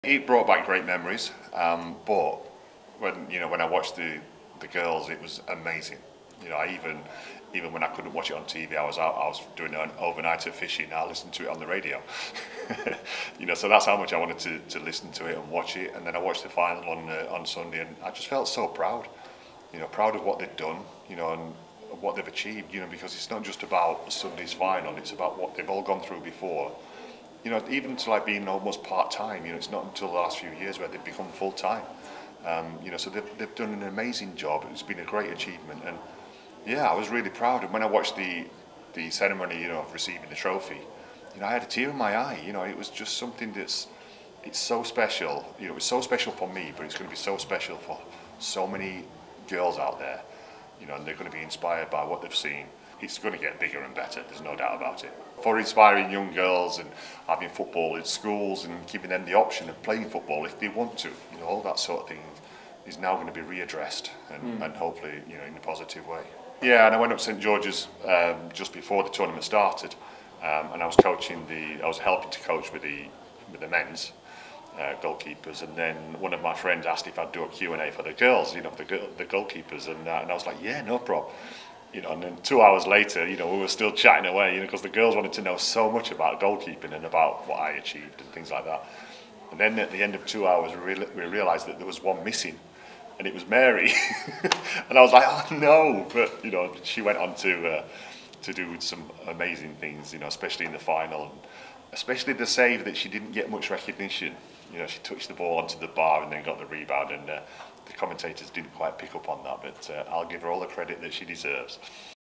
‘I had a tear in my eye!’ | David Seaman ‘so proud’ of Lionesses’ achievement
David Seaman, standing next to his wife (doesn’t talk) Frankie Poultney, discusses England’s victory at the women’s Euro 2022 Final.

The former England goalkeeper, who made 75 appearances for the three lions speaks about how this victory will be inspirational to young girls, and will grow the sport in the UK.

Seaman, who currently coaches at Arsenal, tells PA he had a 2 hour Q&A with the Lionesses’ keepers at St George’s Park before the tournament.

David was attending the National Cat Awards in London as a judge.